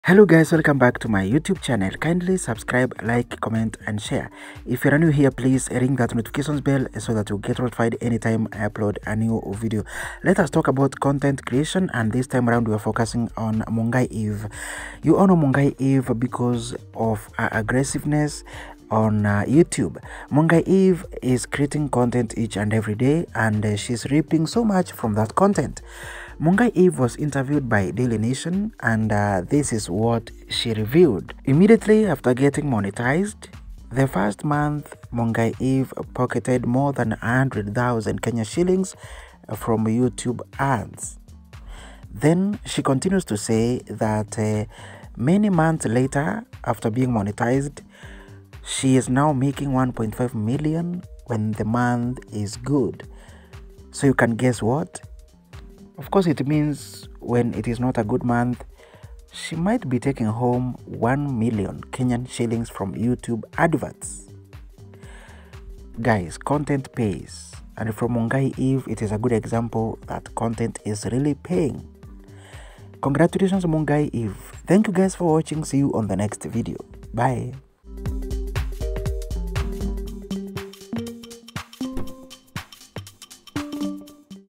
REVEALED: WHAT MUNGAI EVE EARNS ON YOUTUBE MONTHLY
Kenyan YouTuber, Mungai Eve has opened about how much she makes on youtube monthly.